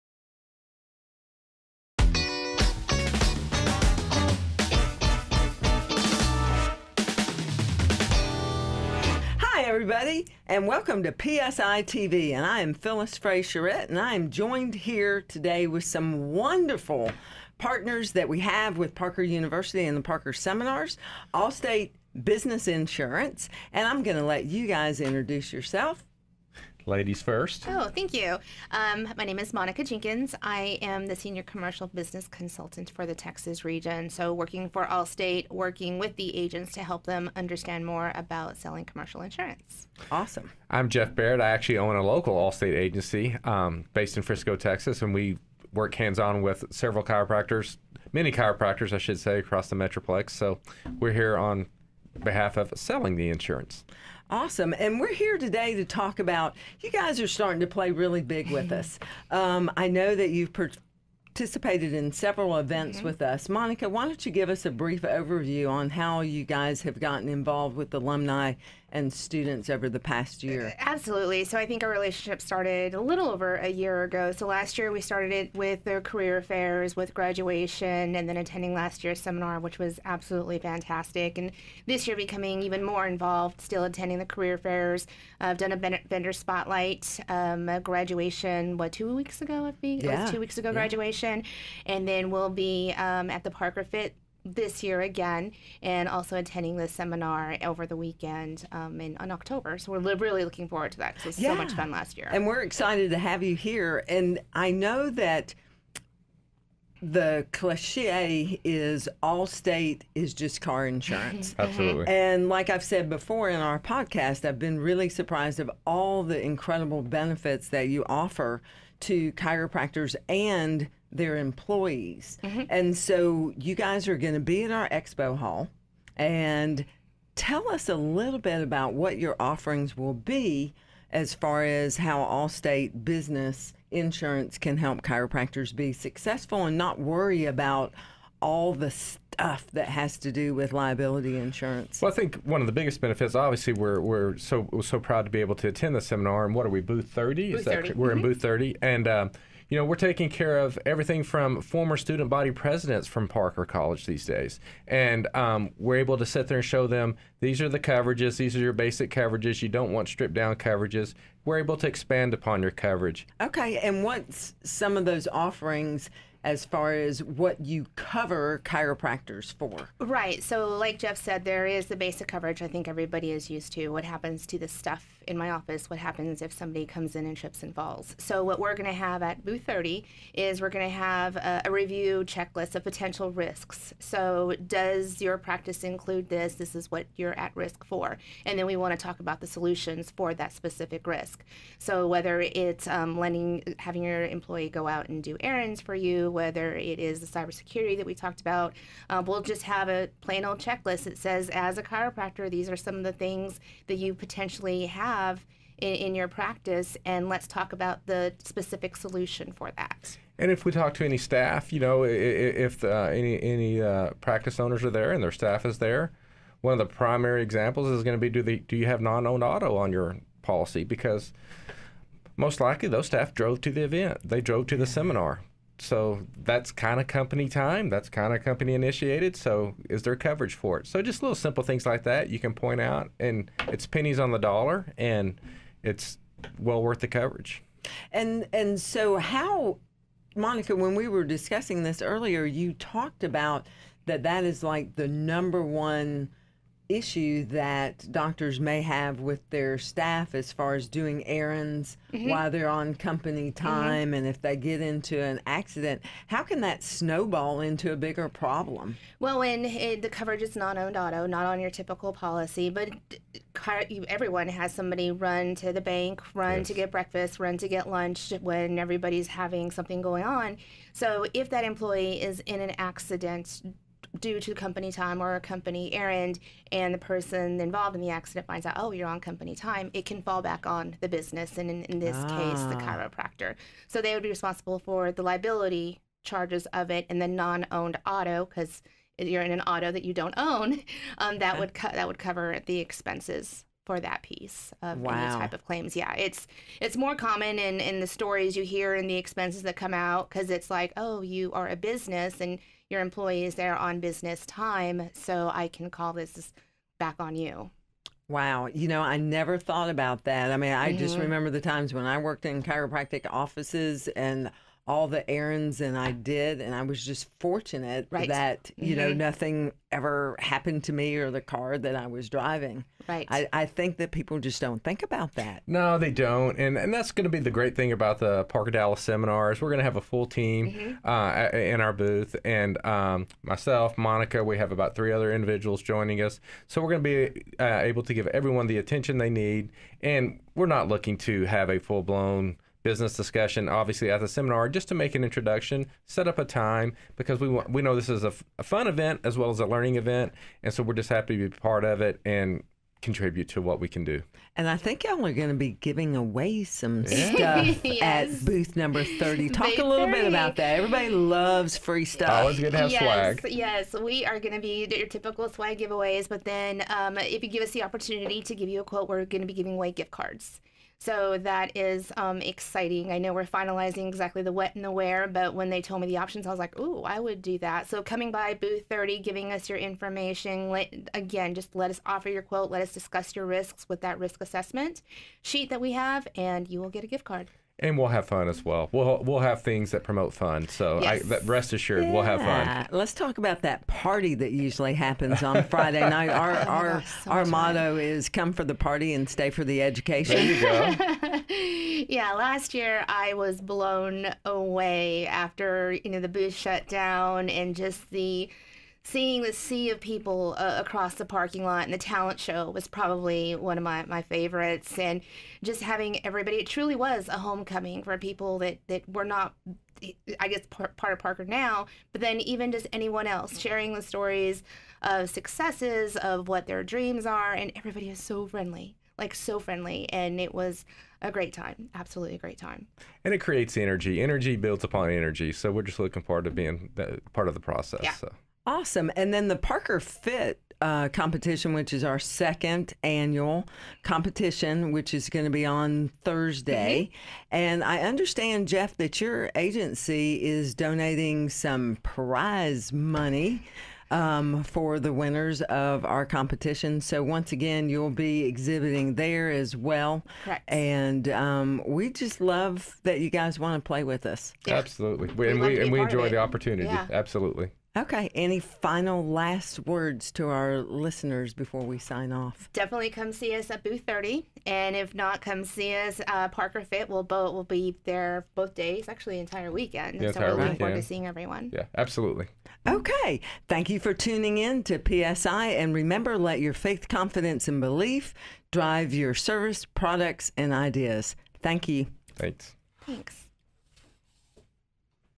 PSI with Phyllis Frase Charrette & Guest, Allstate Insurance (Episode 3)
Listen in as Phyllis discusses how Allstate can help you protect your business from the unique risks that you face as a chiropractor. We have Allstate Senior Commercial Business Consultant, Monica Jenkins, Baird Insurance agent, Jeff Baird, and D. Michell Krenek as our special guests.